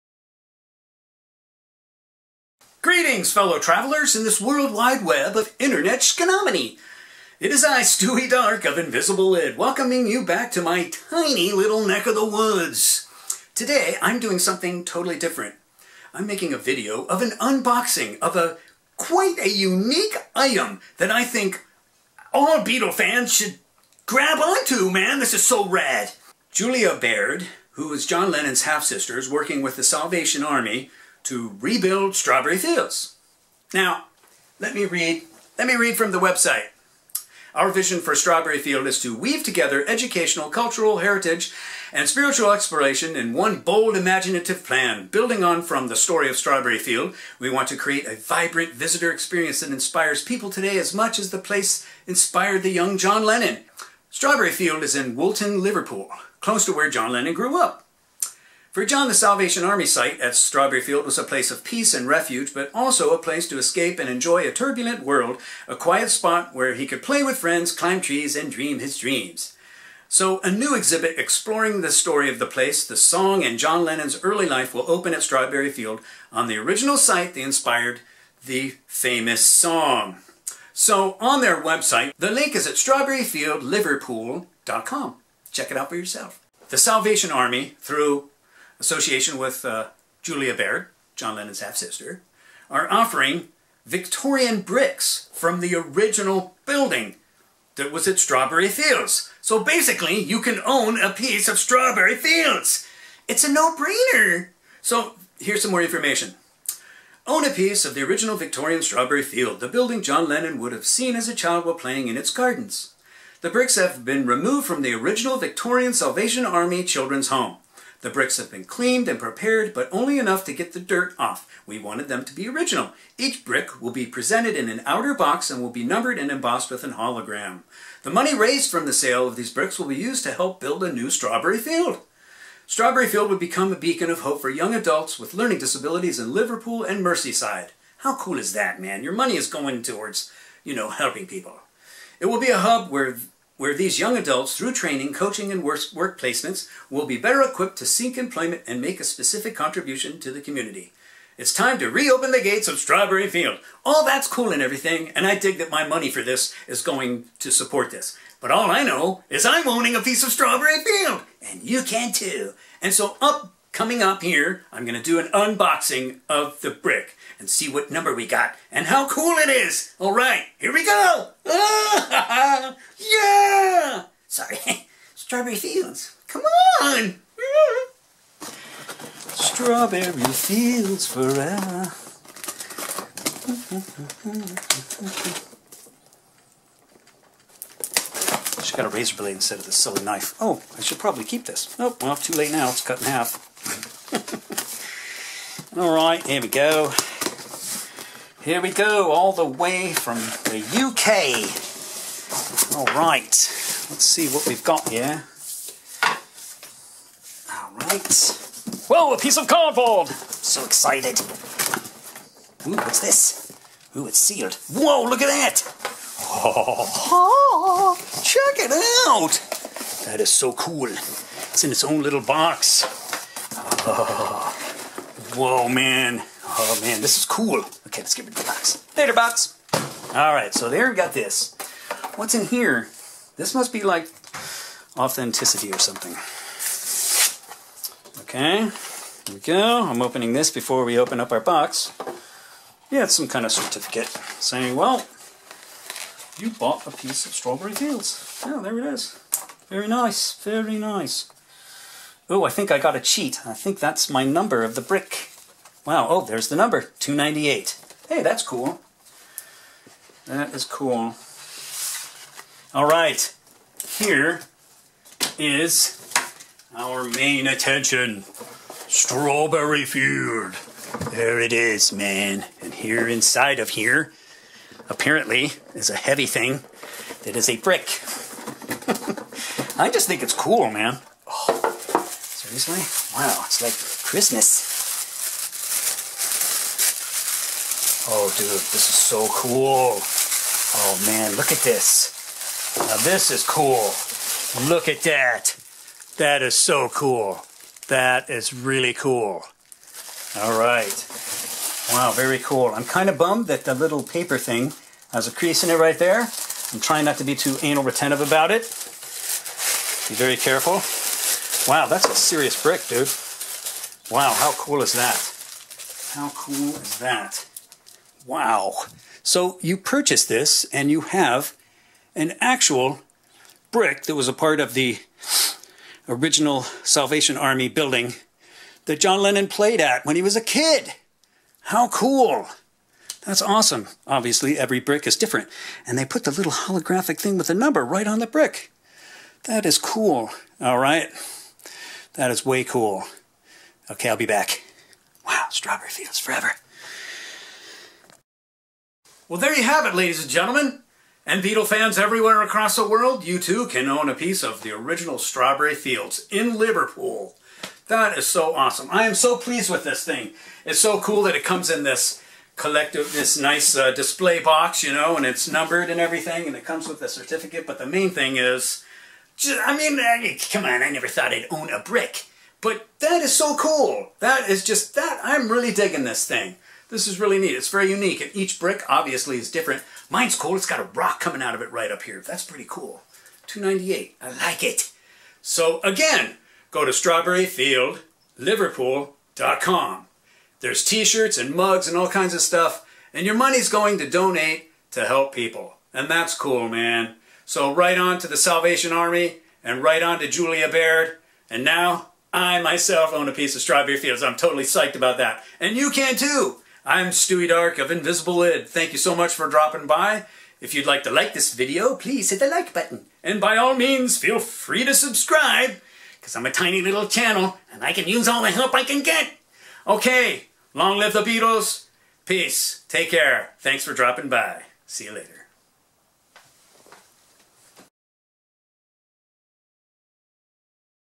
Own a Beatles Strawberry Field Brick - Unboxing
Own a Beatles Strawberry Field Brick - You can skip to 2:59 for the actual unboxing. Through John Lennon's half-sister, Julia Baird, the Salvation Army has started a fund raiser to rebuild Strawberry Field, in Liverpool, UK. The fund raiser offers actual bricks - of 2500 bricks - that came from the original Victorian building at Strawberry Field, where John Lennon played at when he was a child.